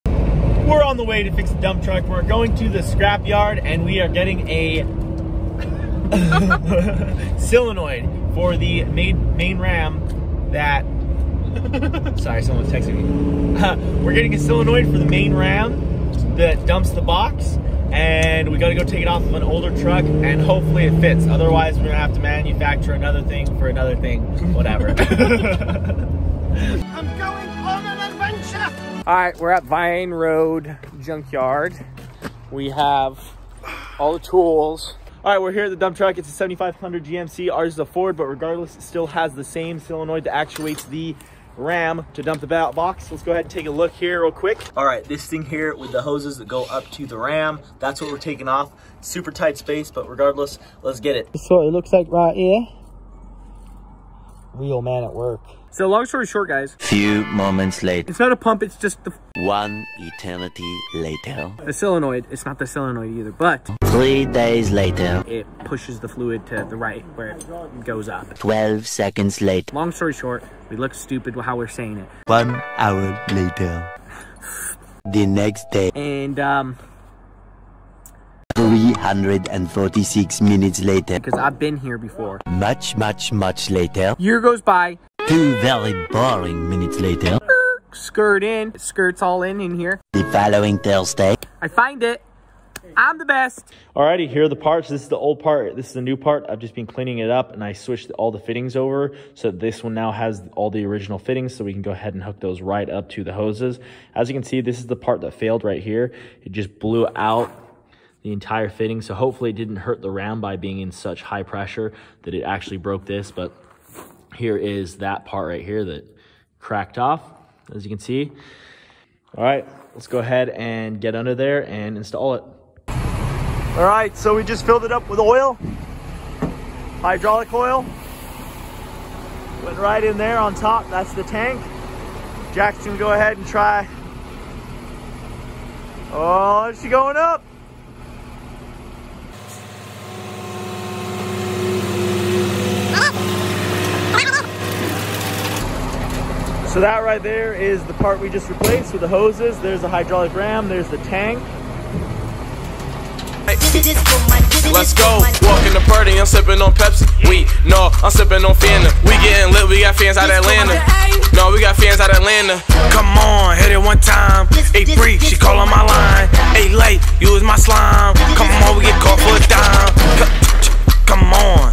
Fixing the old dump truck!
In this episode we fixed a broken valve for the dumpbox hydraulic cylinder. Going to the local junk yard and removing it before swapping the fittings and putting it on our truck. A pretty simple job that ended super well with the dump truck back up and running! This has been an invaluable machine with our goal to build rental properties and we cant go without it for too long.